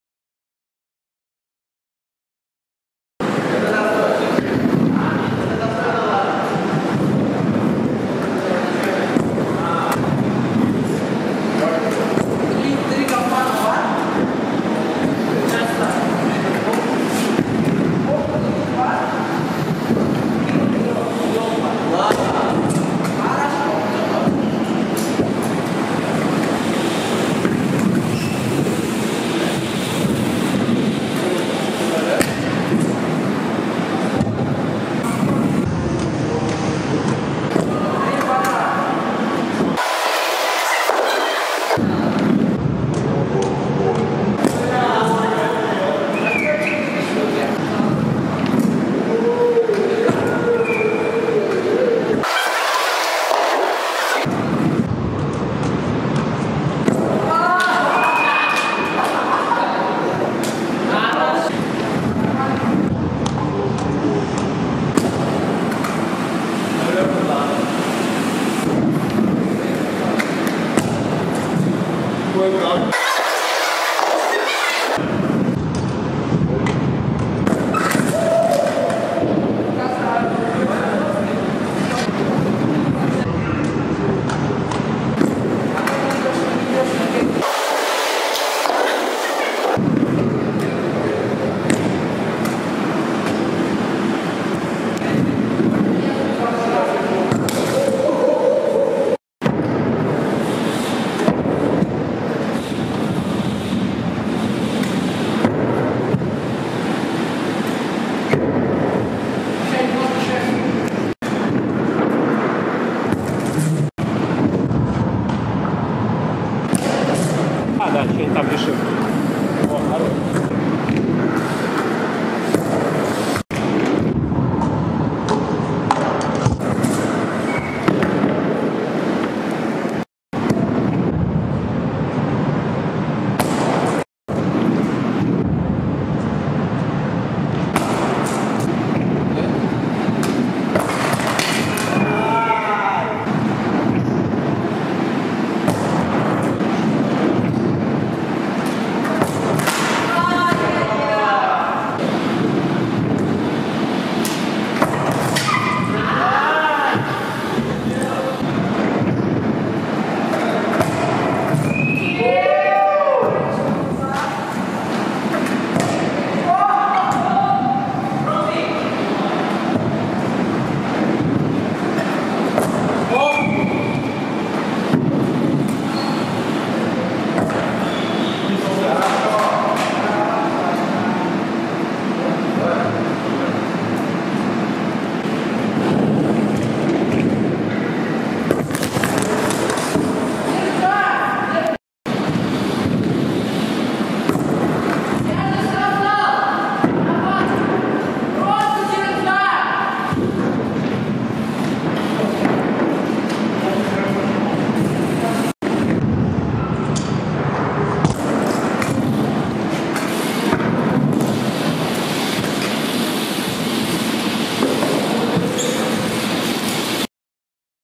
трюки на самокате Ваня Набатчиков @nabat3ikov_ x Rampstroy House skatepark
трюки на самокате, съемка в Москве в скейтпарке RampStroy House м.Шоссе Энтузиастов, 31 с36

Компания РампСтрой строит скейтпарки для проведения соревнований.